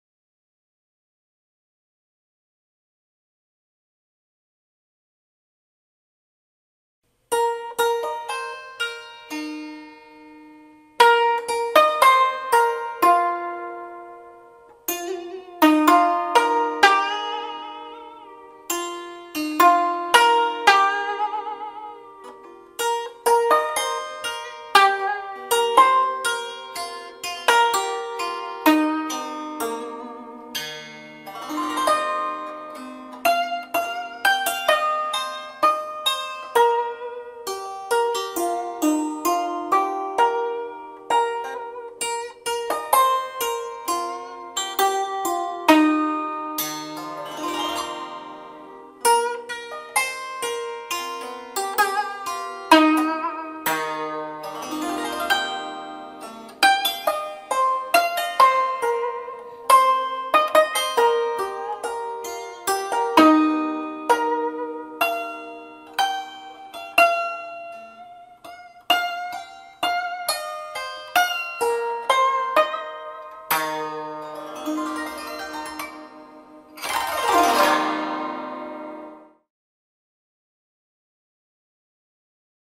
My instruments: The "Tranh"
From time to time I will try to present some selected instruments from my collection. Please keep in mind that I am not a professional performer on these instruments, I just want to give an idea how they sound.

The "Tranh" is a board zither from Vietnam with steel strings. Normally players use artificial nails for playing, like you can see here: